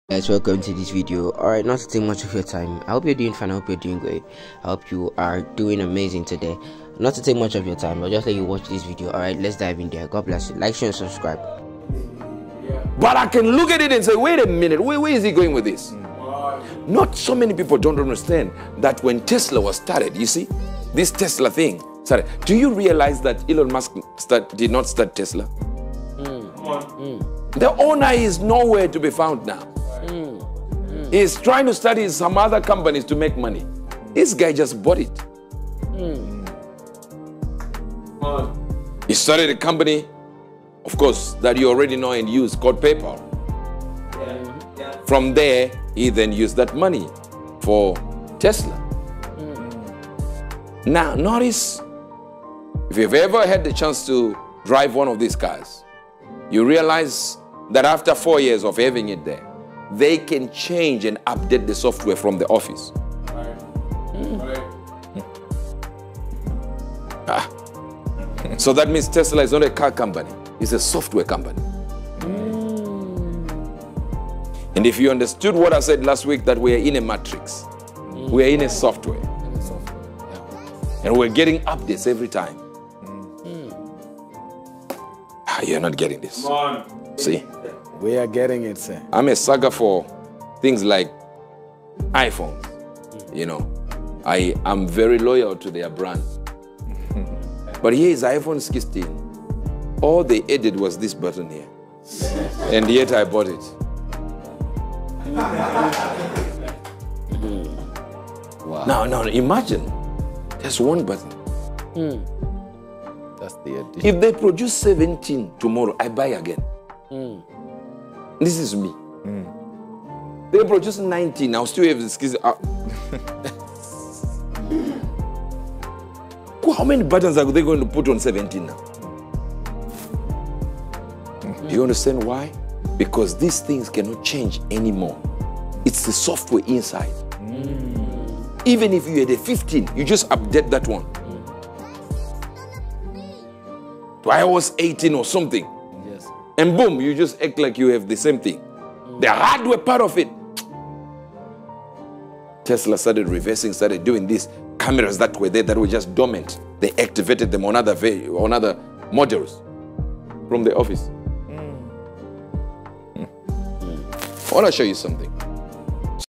Prophet Uebert Angel Gives Reasons Why He Will Buy The iPhone 17 If It Launches 😳😱
Prophet Uebert Angel says he's a loyal lover of the brand iPhone and he gives reason why he'll buy the next iPhone 17 If it's out, even if they don't change anything in the phone